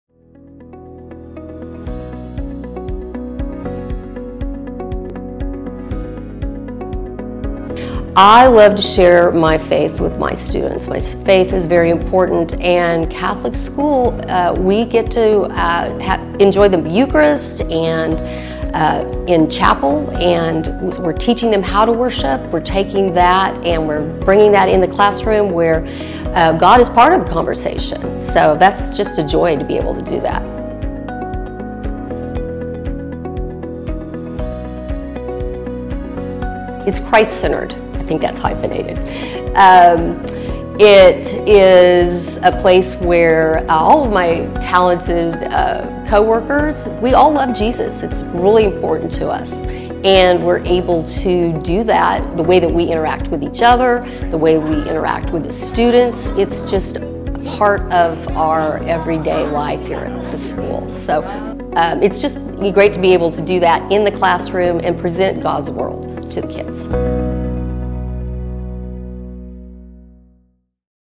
A Moment of Reflection: Karen Fickett
Karen Fickett is a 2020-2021 Cura Personalis Award Winner at Holy Spirit Prep. The Cura Personalis Award, presented at the HSP Annual Gala each year, honors faculty and staff who have supported HSP's Catholic identity and had a significant positive impact on students, parents and colleagues.